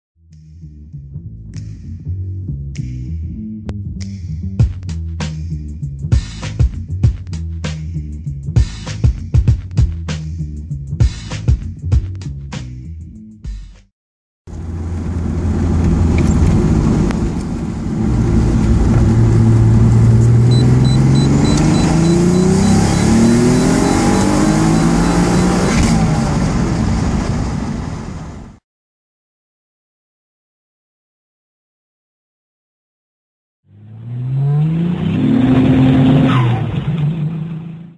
ALan's 1992 Mitsubishi 3000GT VR-4 FULL 2nd Gear Pull @ 24PSI on Pump-E85, BC264's, and 8K RL
**I would NEVER actually start at this low of an RPM lol** After much questioning, I finally tweaked my setup to "tap it out" - until meth is added anyway haha. Enjoy!